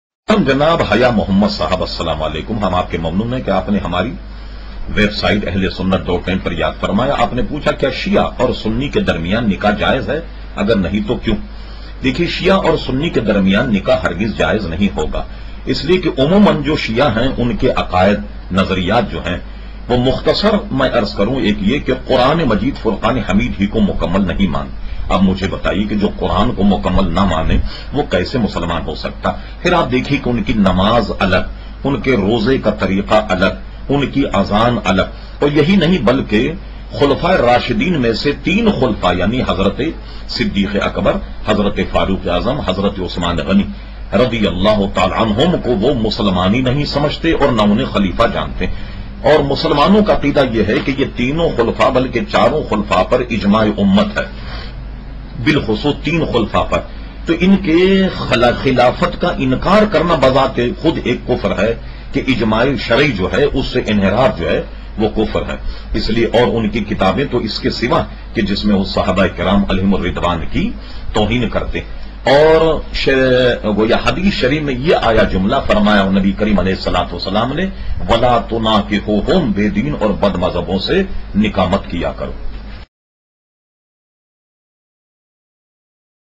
Kia Shia Or Sunni Ke Darmiyan Nikah Jaiz Hai, Agar Nahi To Kiyon? Allama Syed Shah Tarab ul Haq Qadr
Voice:
Allama Syed Shah Tarab ul Haq Qadri (R.A)